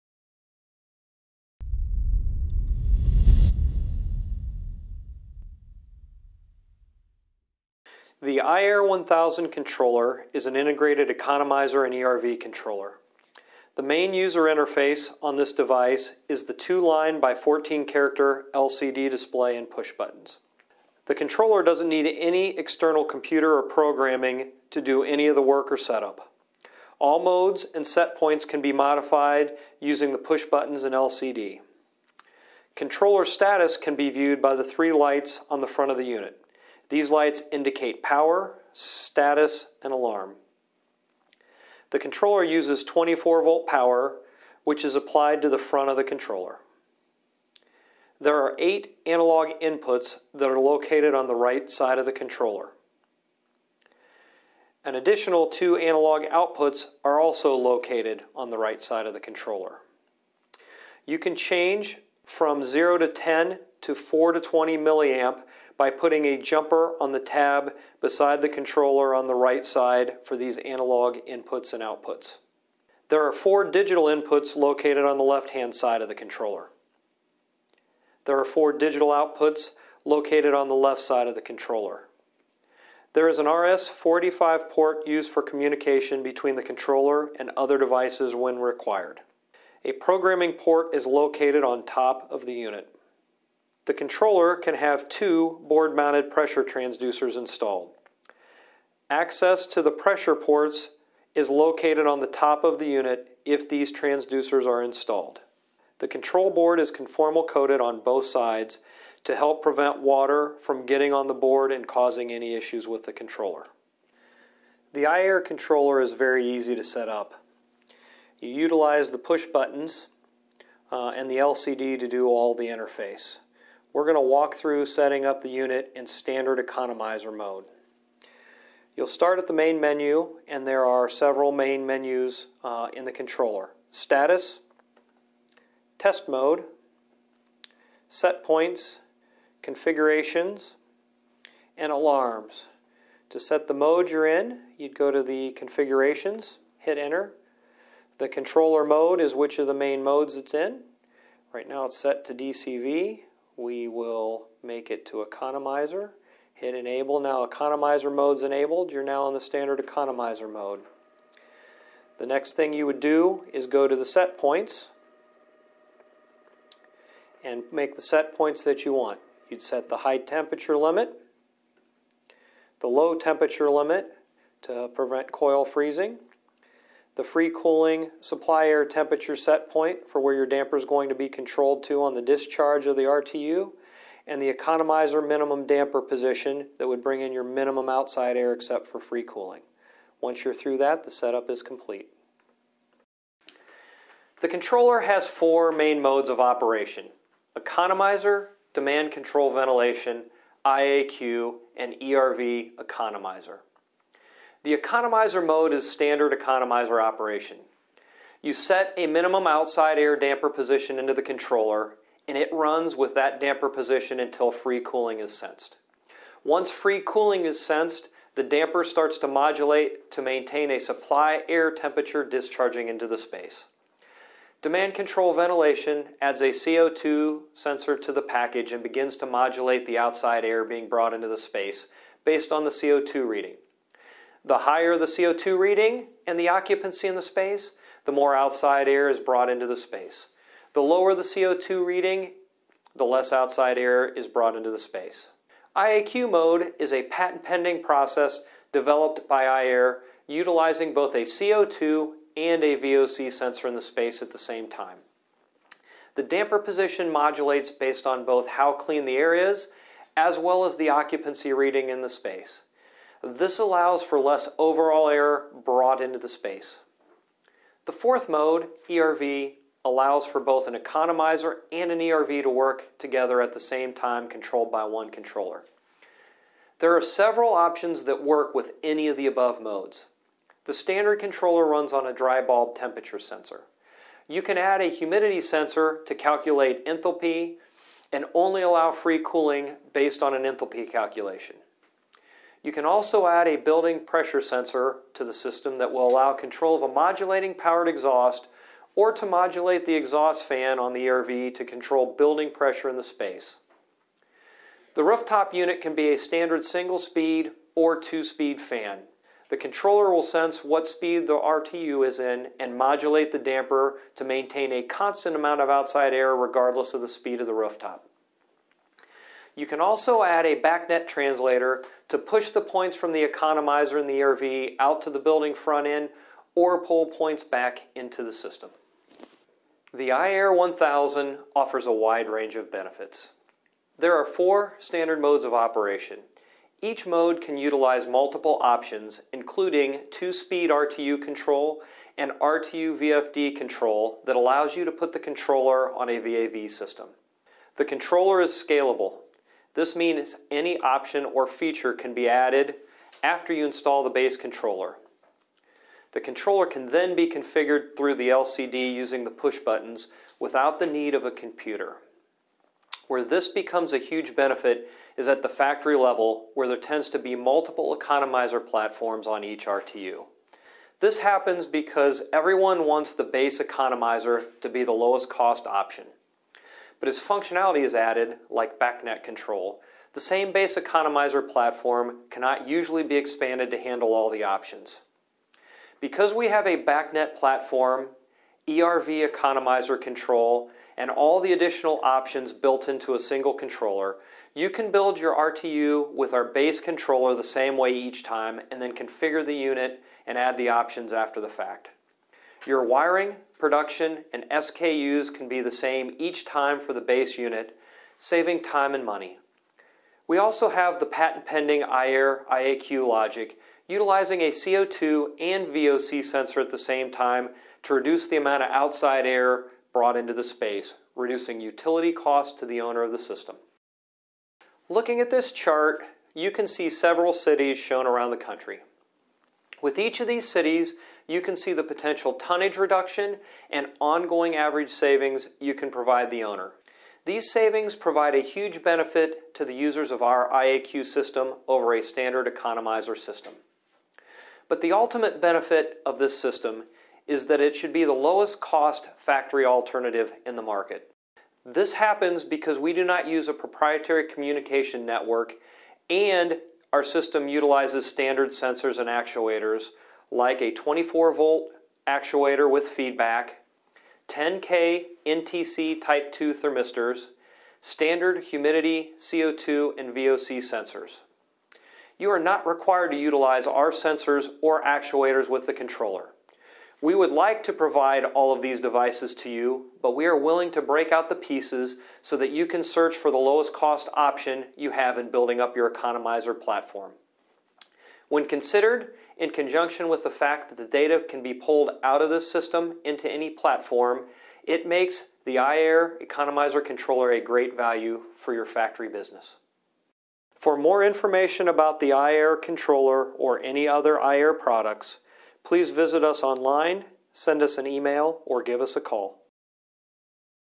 iAIRE economizer controller
Features, benefits and functionality of the iAIRE economizer controller.